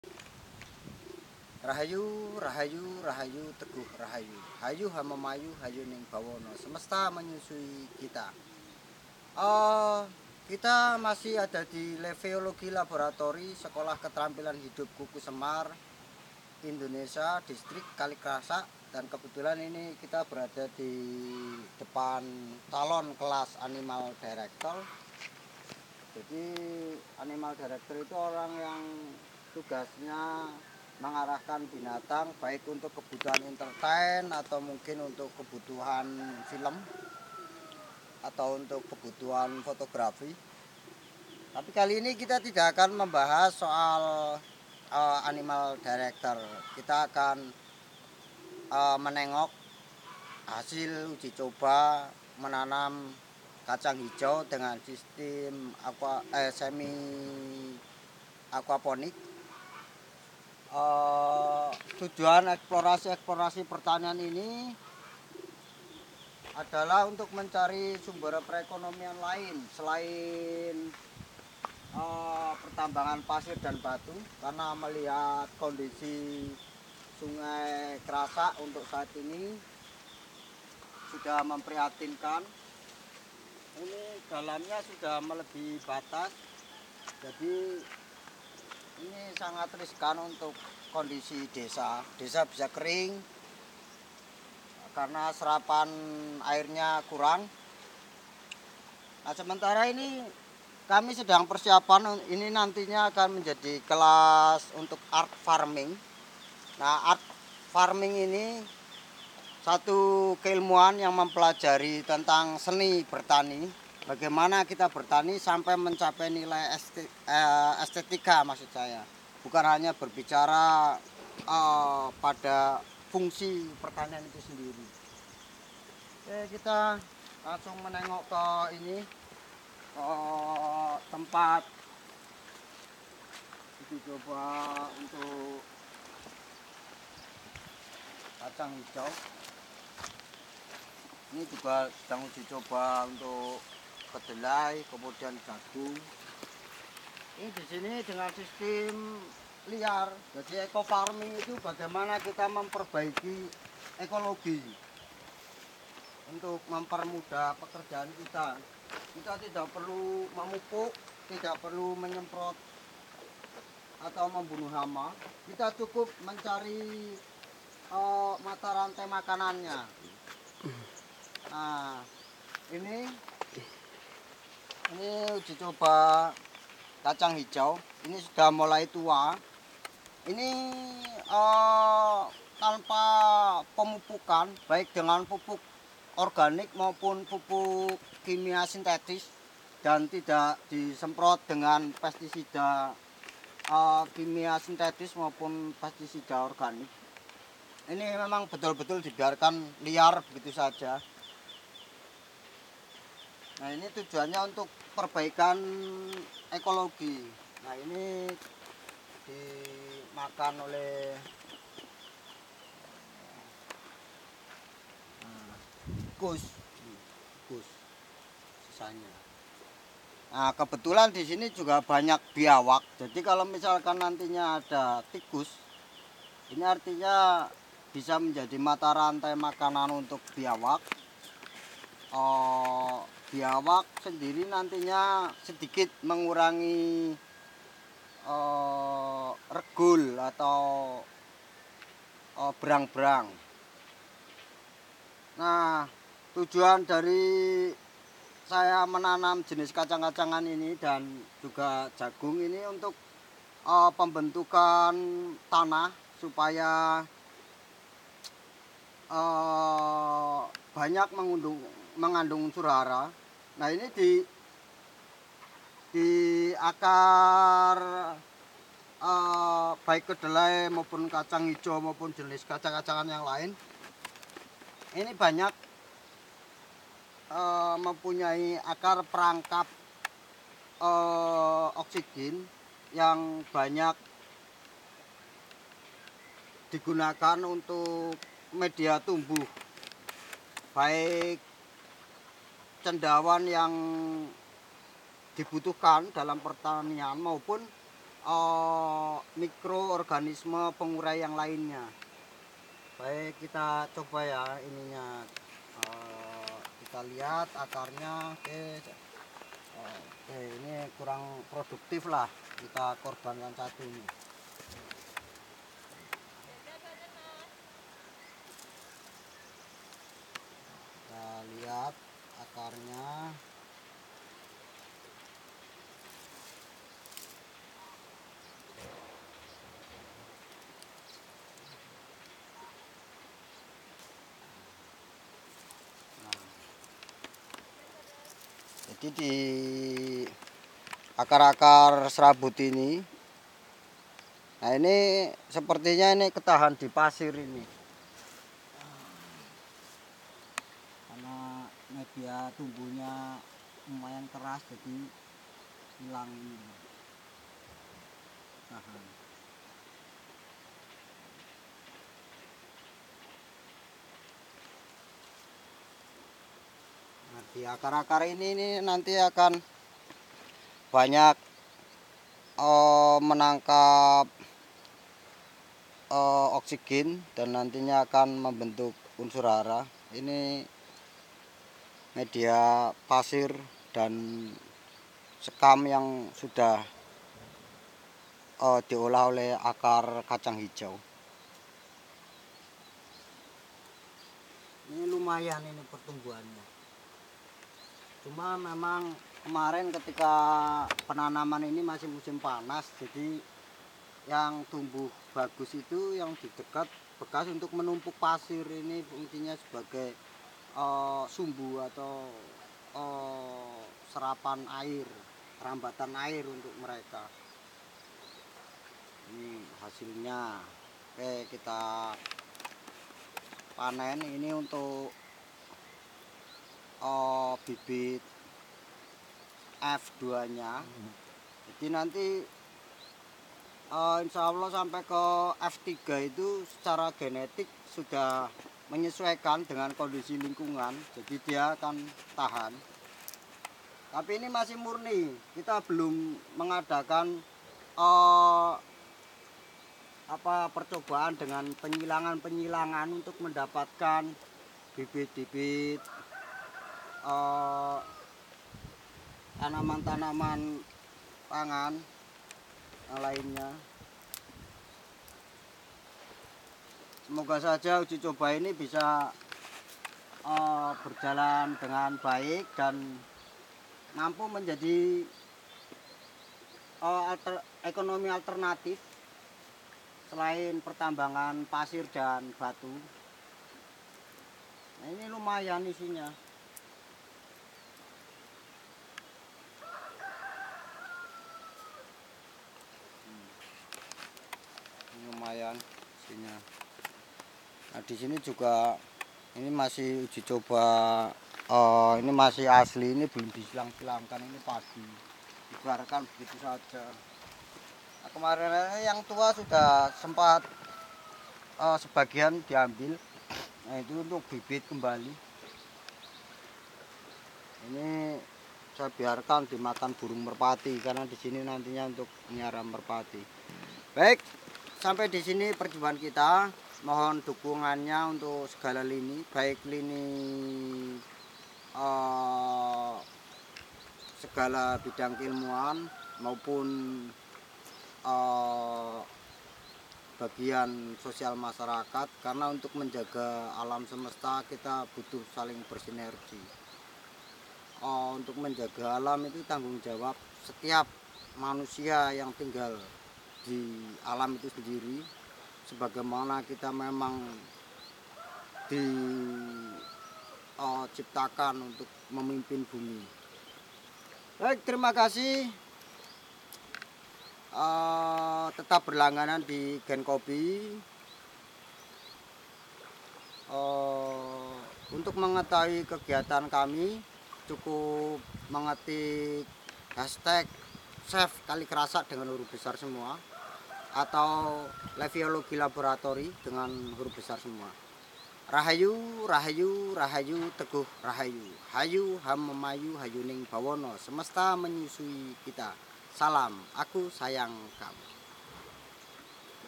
Penjelasan ANIMAL DIRECTOR dan Menengok Uji Coba Pertanian di LIFEOLOGY LABORATORY
Video dokumentasi GENKOBI
GENKOBI sedang berusaha merintis LIFEOLOGY LABORATORY-SEKOLAH KETERAMPILAN HIDUP (SKH)-KUKU SEMAR-INDONESIA -DISTRIK KALI KRASAK.Hal ini salah satu langkah penyikapan GENKOBI ketika melihat kondisi Kali Krasak dan sosial masyarakatnya sangat memprihatinkan,setelah terjadi eropsi tahun 2010 budaya pragmatis menjangkiti sosial masyarakat di sekitar kali krasak,yang dulunya mereka memiliki budaya pertani dan membuat gula merah dari kelapa,sekarang mereka beralih pada pertambangan pasir dan batu.Sedang kondisi sungai (yang dulunya lahan pertanian) kini sudah terlalu dalam.